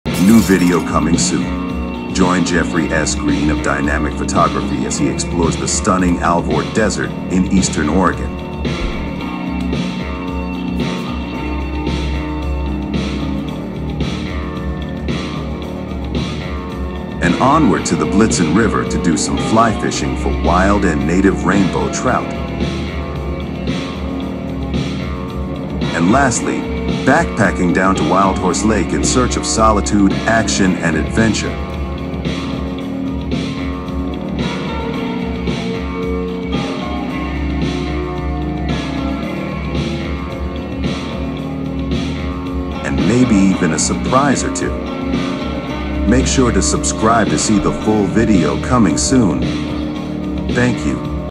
Alvord Desert, Blitzen River, & Wildhorse Lake in Oregon! New Video Link Below.
NEW Video Coming Soon! Alvord Desert, Blitzen River, & Wildhorse Lake in Oregon!
Exploring the Steens Mountain in Eastern Oregon!